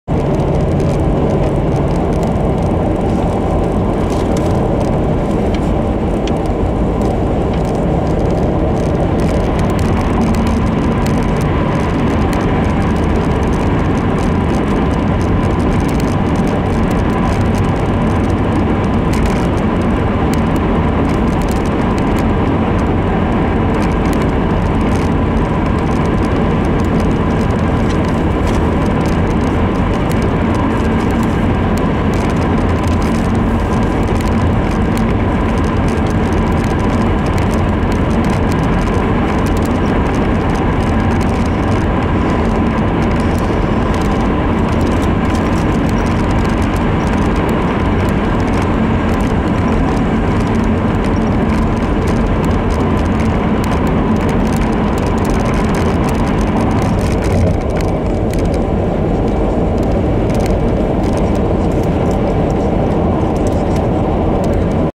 180101_大長瀬TN[E10東九州道-北九州JCT方面]
東九州自動車道を、蒲江インターチェンジ方面から佐伯堅田インターチェンジ方面へ走行。大長瀬トンネルを北方向に通過。